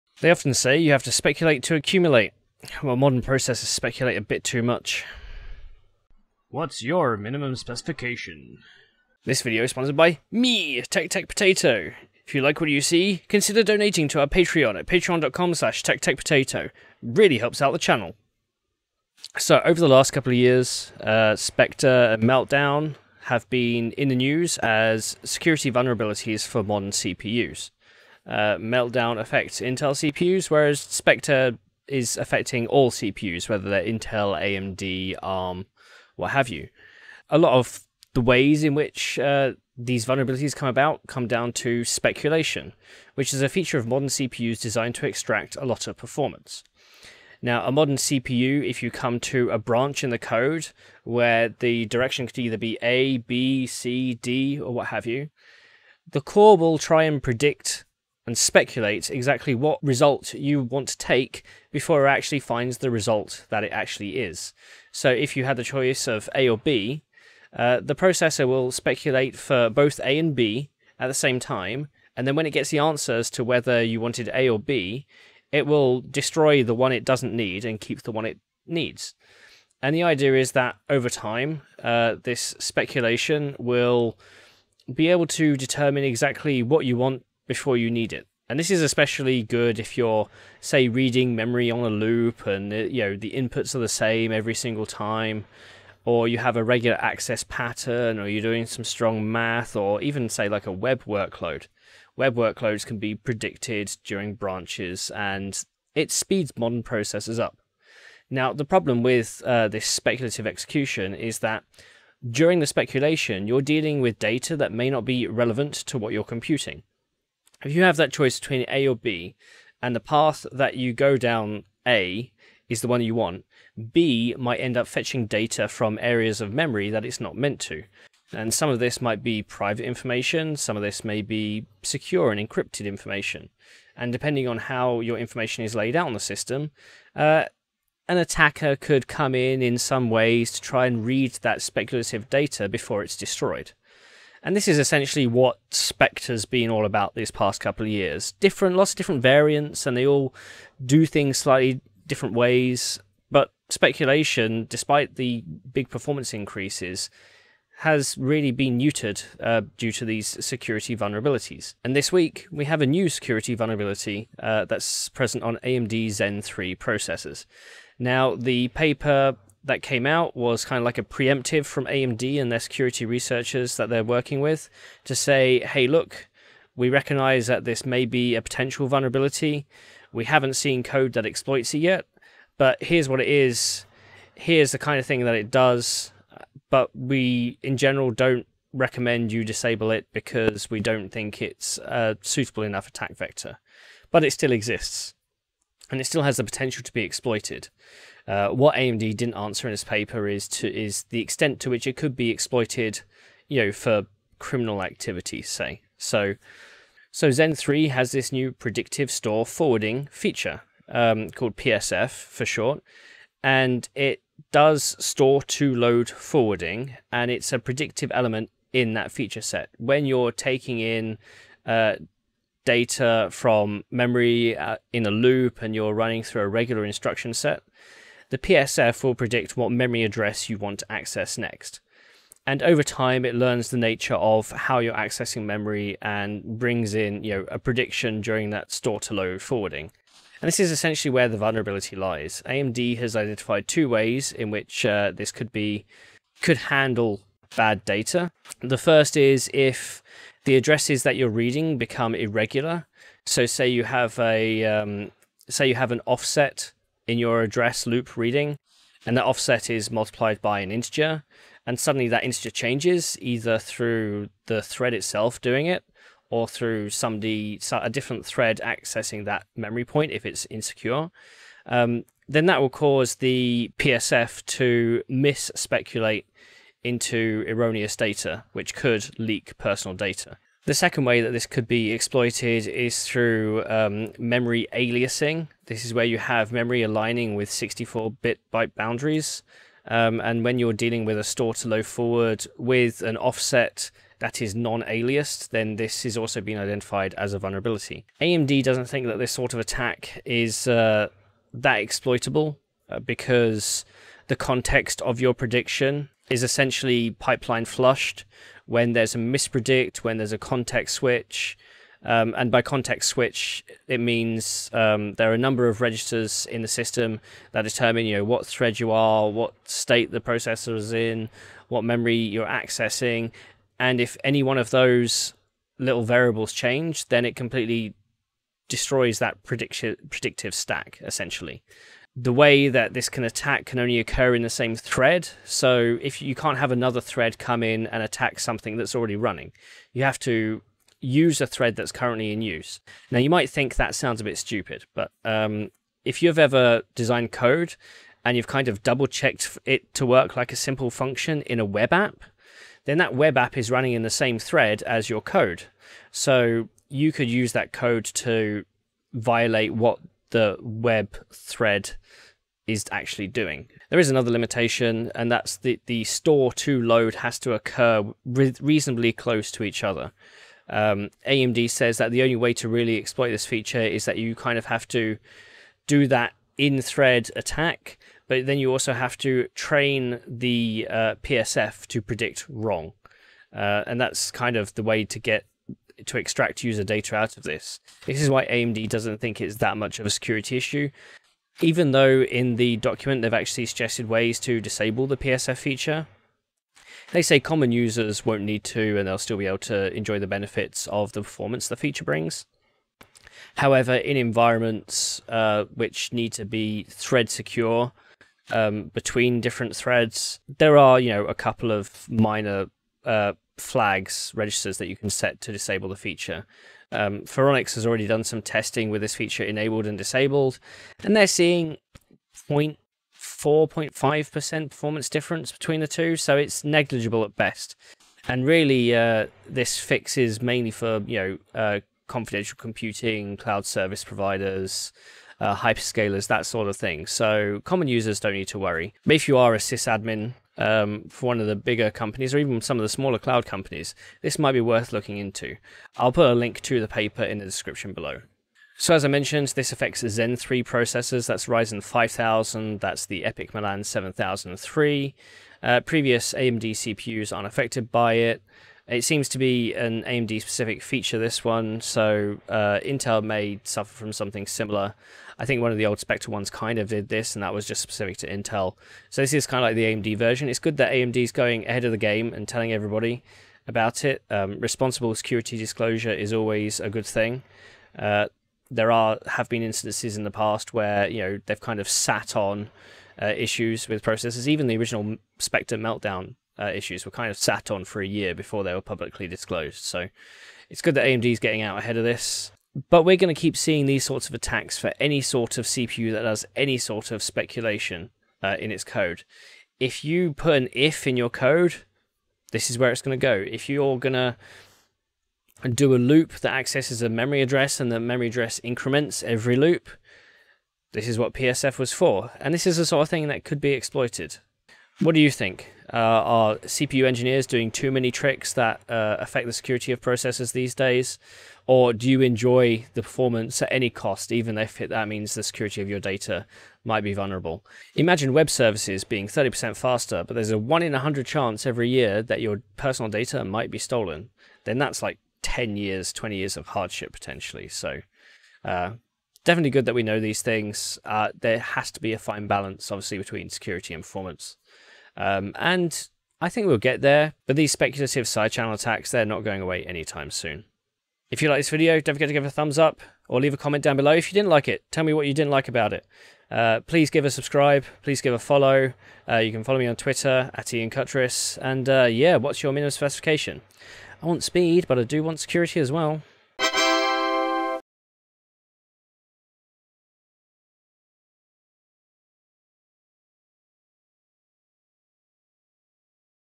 What's Your Minimum Speculation?!
Everyone loves Spectre and Meltdown. We were overjoyed as AMD has just disclosed another vulnerability, but thankfully it's minor.

Timestamps

0:00 Intro
0:19 Spectre and Meltdown
0:42 Speculation and Security
2:41 AMD Predictive Store Forwarding
3:26 Store to Loads
4:08 First issue
4:40 Second issue
4:58 Limitations of a PSF Attack
6:28 Mitigations
6:57 Performance Effect
7:29 Zen 3 affected
8:13 These attacks won't end
9:27 CAT TAX

Links

Many thanks to our CPU CEO Patrons, including:
- Michael Spencer